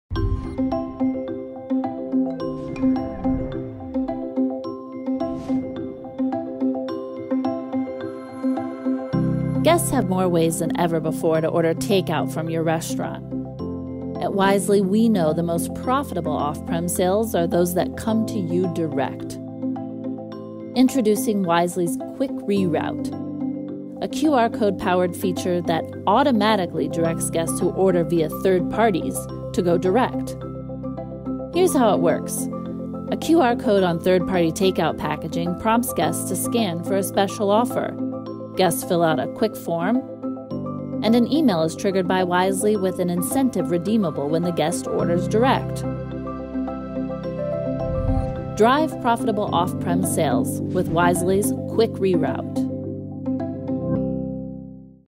Wisely | Quick Reroute
Wisely’s answer for driving profitable off-prem sales—rerouting guests who order via third party to order direct with a QR code powered automated email & offer